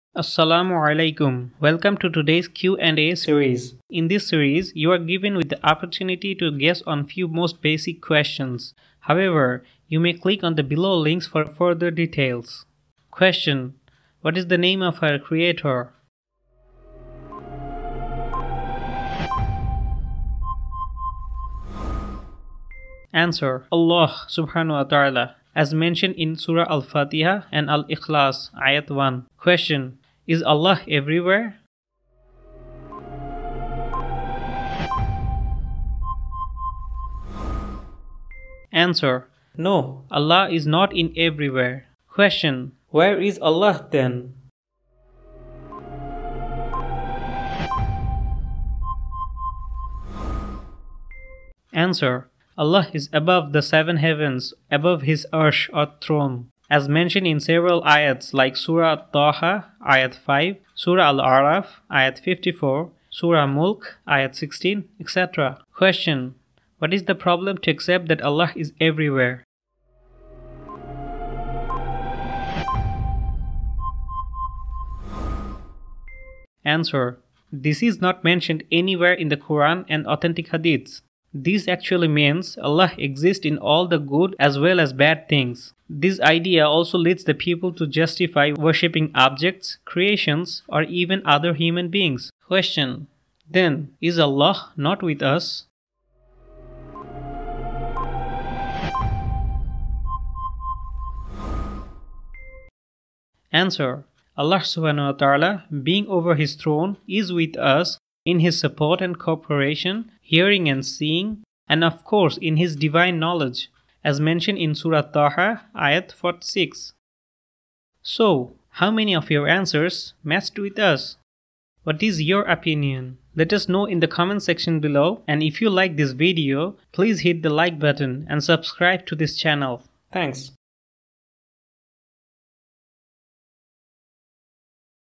Islamic General Knowledge | QNA Part 1 - Introducing Our Creator
Islamic Basic/General Knowledge | Where is Allah? Is God Everywhere or Arsh/Throne | Idea of our Creator/Allah

Muhammad(pbuh) had to go up in Mi'raj, crossing seven Heavens, to reach out to Allah(swt).

We go to Shejdah and say - ‘Subhaana Rabbiyal A’ala - means Glory be to Lord, the Most High

Welcome to today's Q & A series. In this series, you are given with the opportunity to guess on few most basic questions.

1. Q: What is the name of our Creator?
Answer: Allah(swt) (As mentioned in Surah Al-Fatiha and Al-Ikhlas: Ayat 1)

II. Q: Is Allah everywhere?
Answer: No, Allah is not in everywhere.

3. Q: Where is Allah then?
Answer: Allah is above the seven heavens, above His 'Arsh or Throne. (As mentioned in several Ayats like Surah Taha Ayat 5, Surah Al-Araf Ayat 54, Surah Mulk Ayat 16, etc.)

4. Q: What is the problem to accept that Allah is everywhere?
Answer: This is not mentioned anywhere in the Quran and authentic Hadiths. This actually means, Allah exists in all the good as well as the bad things. This idea also leads the people to justify worshiping objects, creations or even other human beings.

5. Q: Then, is Allah not with us?
Answer: Allah(swt) being over His Throne is with us in His support & cooperation, hearing & seeing; and of course in His divine knowledge. (As mentioned in Surah Taha Ayat 46)

Thanks